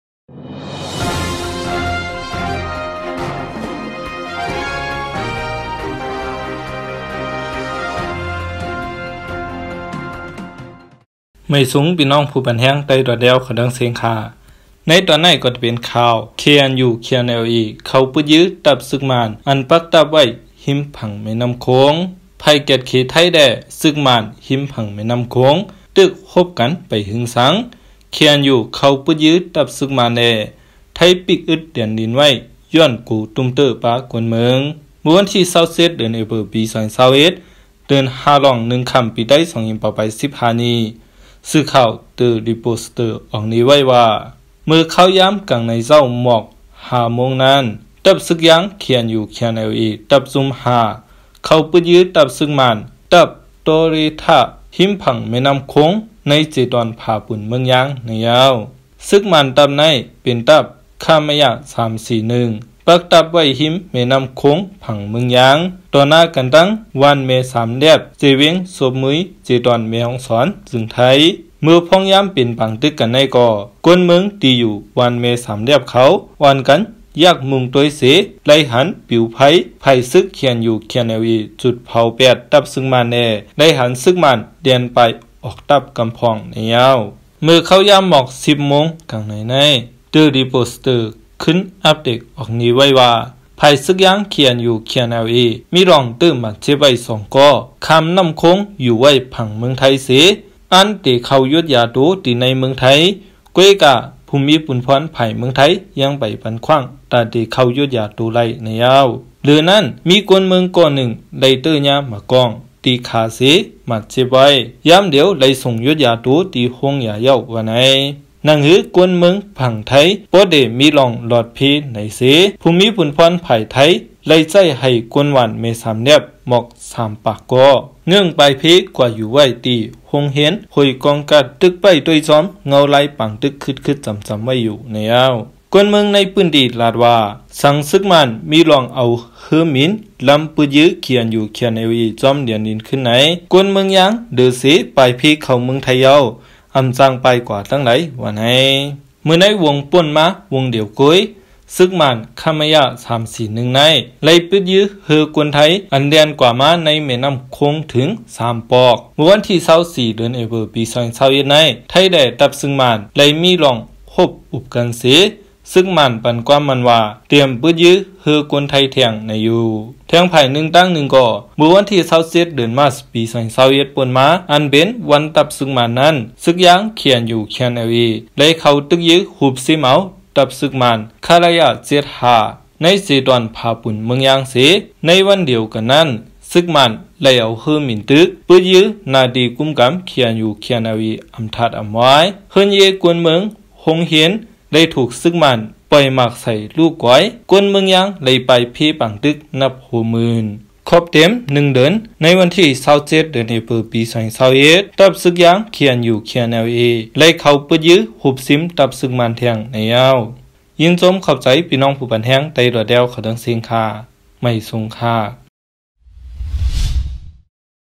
TaiFreedom Video 27.04.2021 KNU/KNLA ၶဝ်ႈပိုတ်းယိုဝ်းတပ်ႉသိုၵ်းမၢၼ်ႈ
TaiFreedom Video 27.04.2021 KNU/KNLA ၶဝ်ႈပိုတ်းယိုဝ်းတပ်ႉသိုၵ်းမၢၼ်ႈ ဢၼ်ပၵ်းတပ်ႉဝႆႉ ႁိမ်းၽင်ႇမႄႈၼမ်ႉၶူင်း